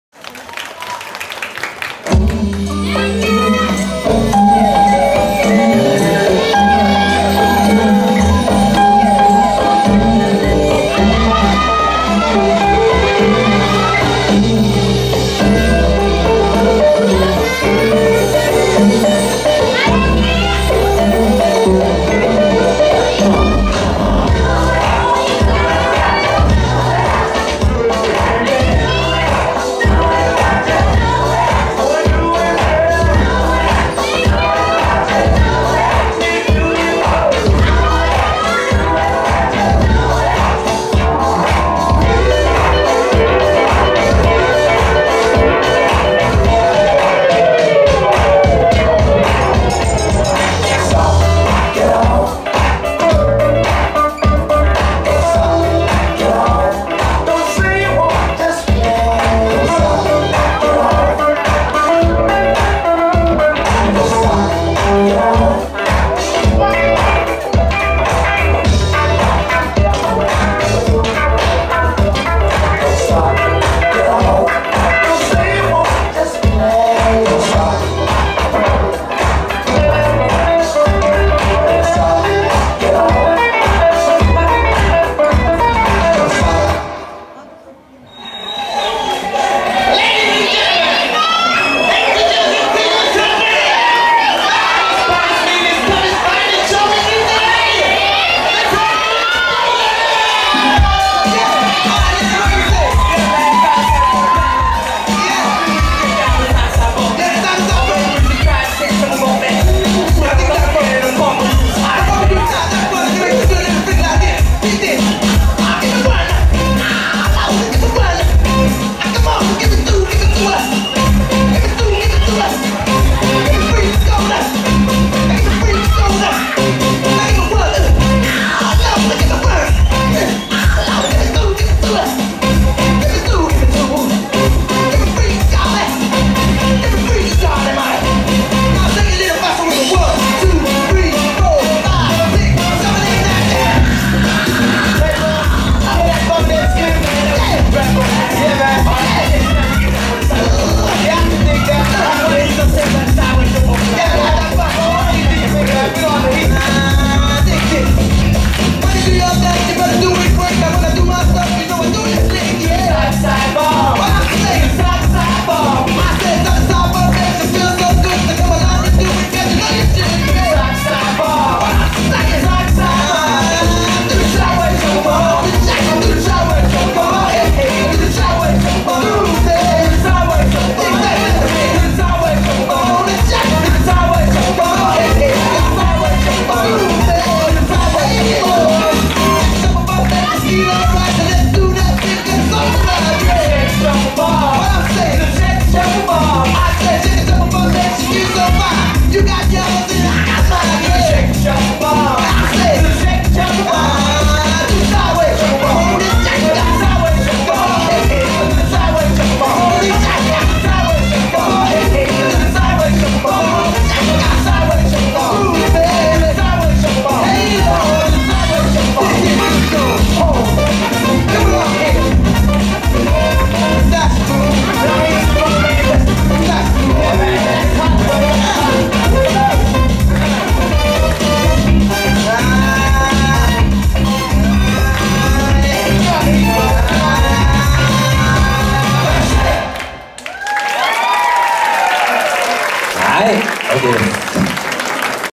GET FANCY（Team Scooby-doo）２－１０ HOT PANTS VOL.34 2015/3/8(sun)
３ヶ月に１度 おこなわれている、ショーケース　メインのオールジャンルダンスイベント「HOT PANTS」!!
毎回　豪華なゲストダンサーや、発表会ナンバー、一般チームが　６０を超え出演するビックイベント、　クラブタイムでは、HOT PANTS専属のDJ、そして　とびきりのゲストDJをお招きして、お客さん、出演者、みんなでがっつり楽しんでます！
毎度　かなりの盛り上がりをみせる、アットホームなビックパーティです（^o^）